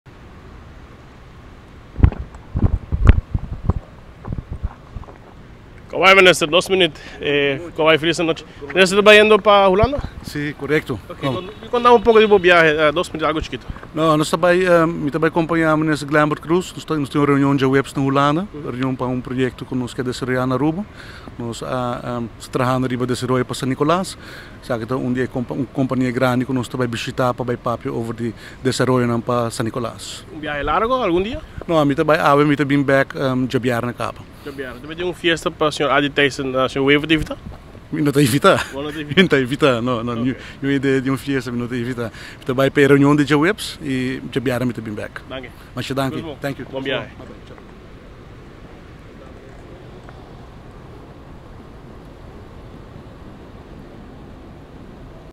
No a invit’e p’e fiesta: Minister Wever biahando pa trabou
Diamars anochi a tuma nota presencia di Minister Geoffrey Wever na aeropuerto internacional Reina Beatrix. A acerca e mandatario pa puntr’e riba e motibo di su presencia, claro; e ta bay biaha pero e motibo di su biahe y su destinacion tabata desconoci. Wever di un forma masha habri a contesta cu e ta bayendo Hulanda den cuadro di un reunion relaciona cu desaroyo di San Nicolas.